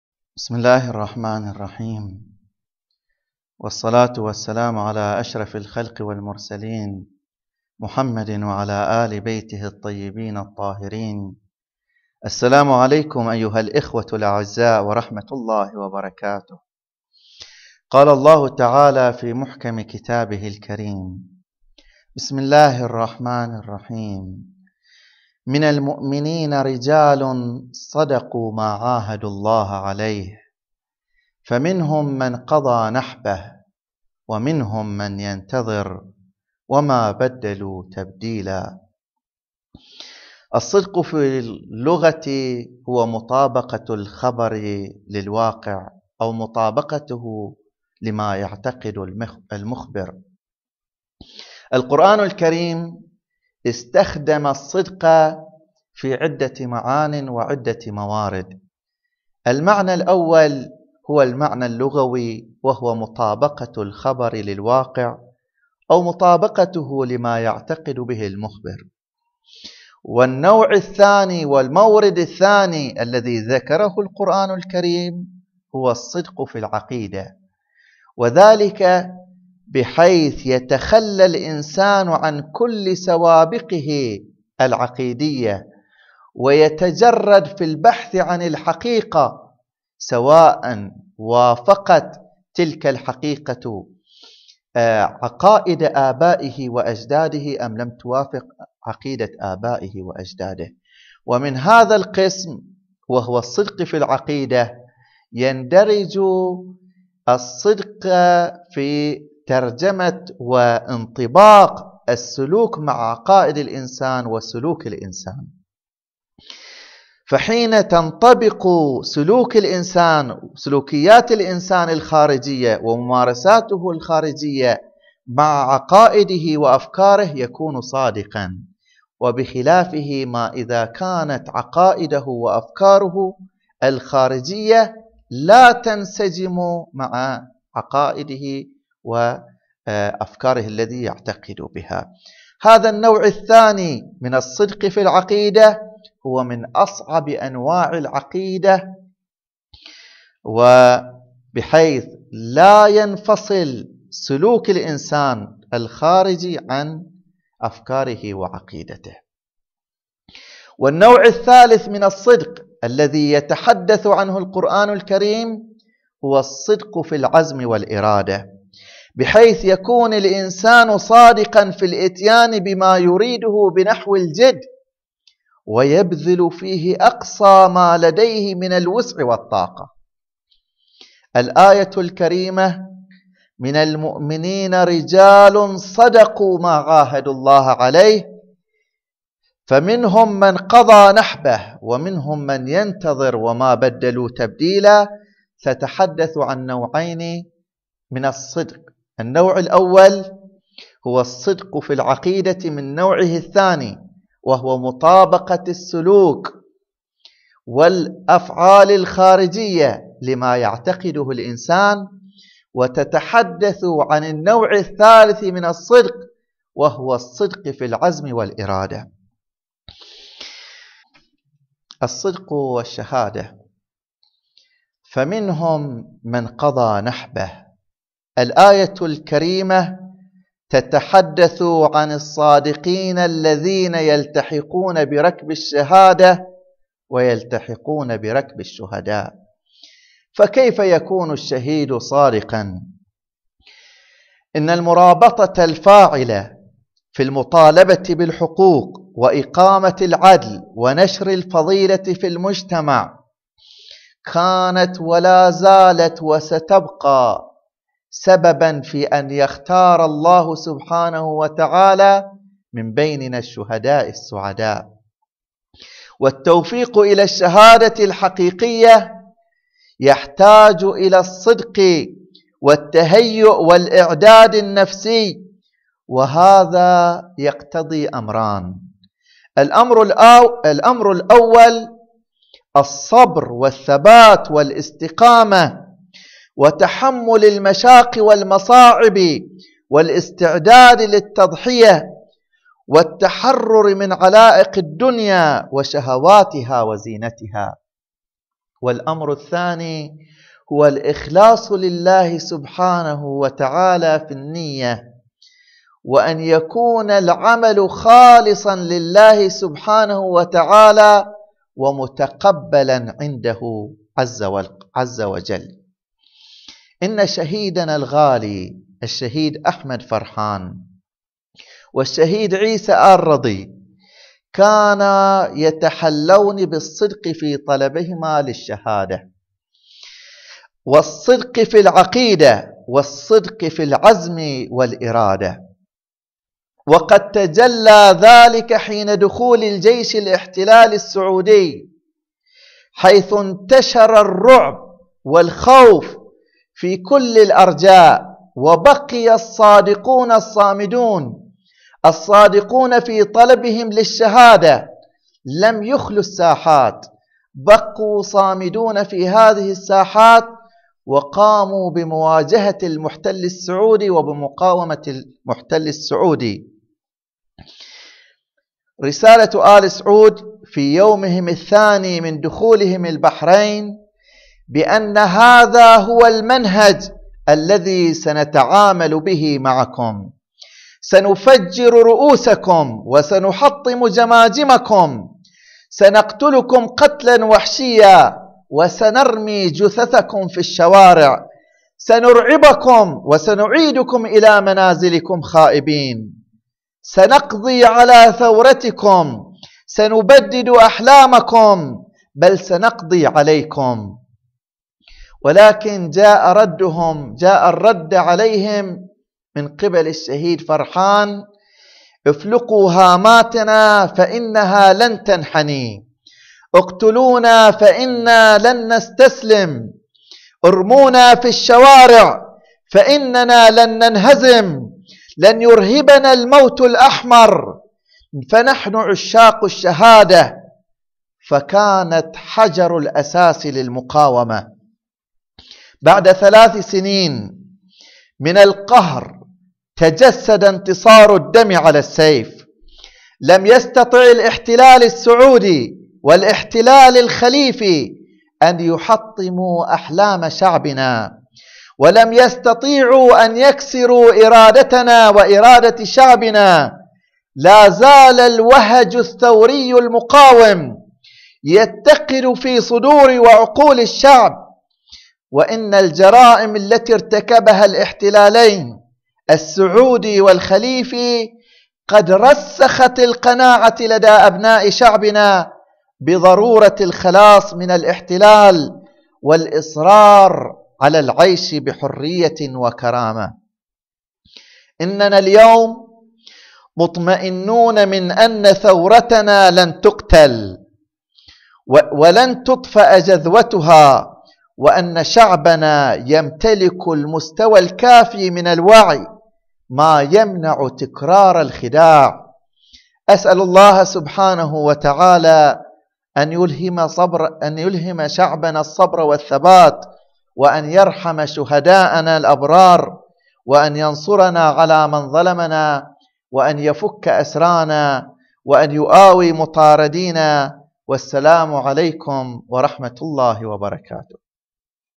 كلمة السيد مرتضى السندي - التأبين المركزي لشهداء مجزرة سترة Bahrain
كلمة القيادي في تيار الوفاء الإسلامي "السيد مرتضى السندي" في التأبين المركزي لشهداء مجزرة سترة والذي نظمه تيار الوفاء الإسلامي مساء اليوم الأثنين (17 مارس/آذار 2014) بمنطقة مهزة جنوب العاصمة البحرينية المنامة تخليداً لذكرى الشهيدين "أحمد فرحان، عيسى آل رضي" الذين استشهدوا بأيدي قوات الإحتلال "السعودي الإماراتي" في الخامس عشر من شهر مارس 2011.